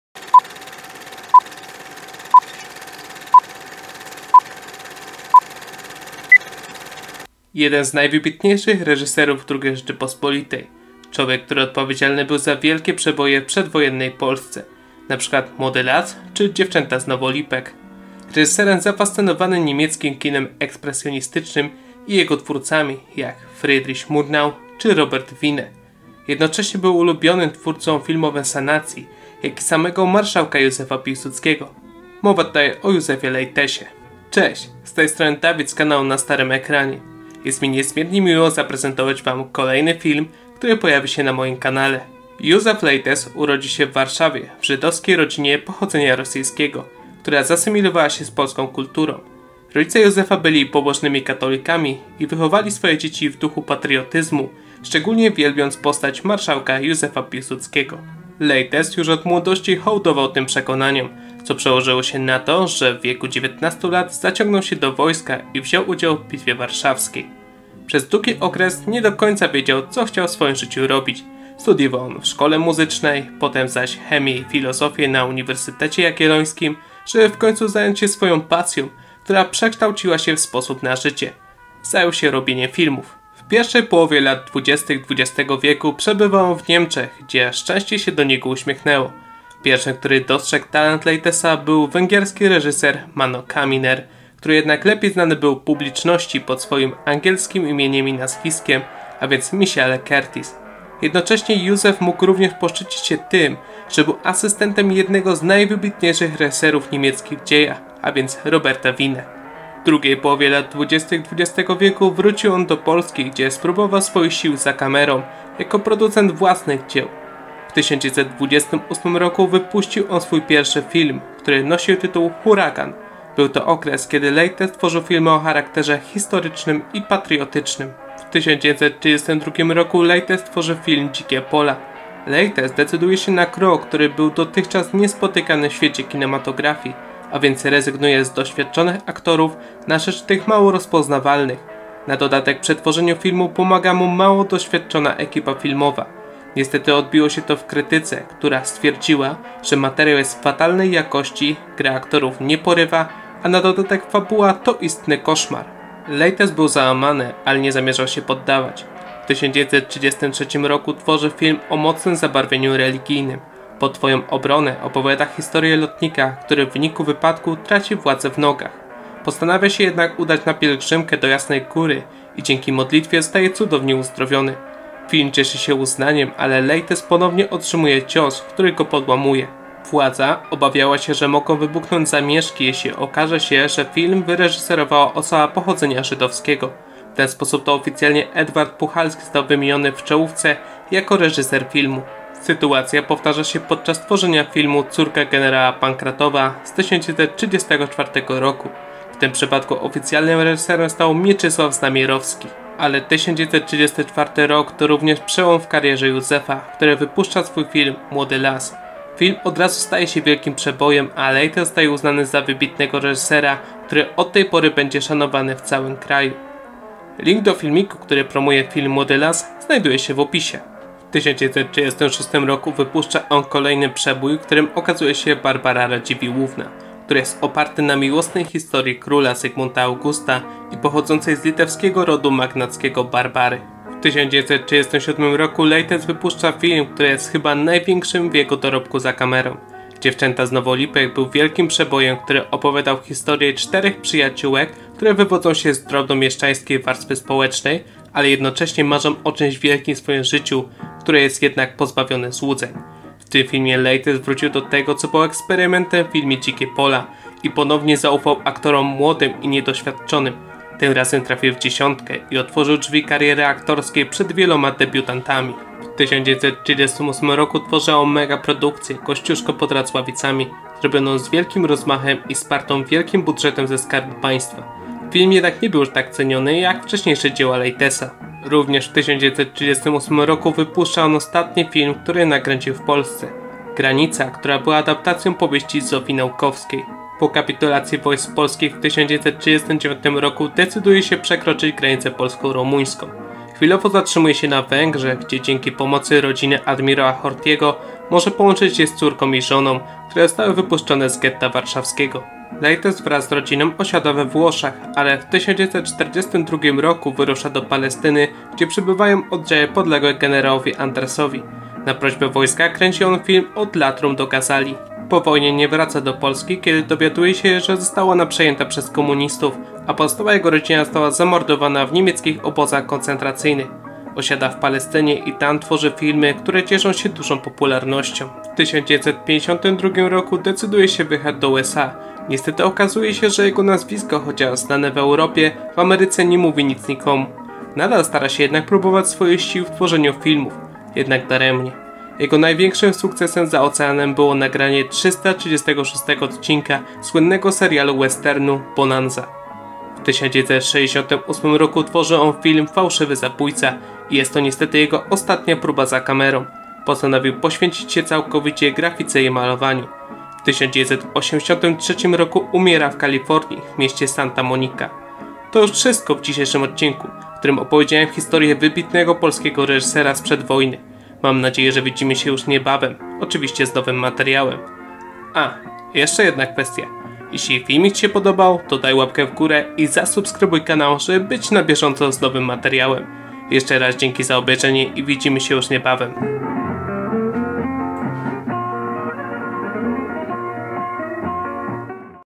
Patriota, katolik, Żyd i ulubiony reżyser Piłsudskiego!
Józef Lejtes był jednym z najwybitniejszych reżyserów doby II RP. Nakręcił takie filmy, jak "Młody las", "Dziewczęta z Nowolipek" czy "Kościuszko pod Racławicami". Jego filmy zyskiwały nawet w oczach władzy sanacyjnej i Marszałka Piłsudskiego. Niestety, ale wojna spowodowała, że dzisiaj w kraju mało kto o nim pamięta...

Pamiętaj, że subskrypcja, łapka w górę lub komentarz jest mile widziane:)

Link do filmiku, który promuję film "Młody Las" - Życzę miłego oglądania

PS: Jeśli widzisz w filmiku zielone światełka, które pojawiają się co jakiś czas, to przepraszam cię z góry. Niestety nie mam na to wpływu, a podczas tworzenia filmiku, nie występują takie błędy. Mogę dodać tylko od siebie, że to złośliwość rzeczy martwych:(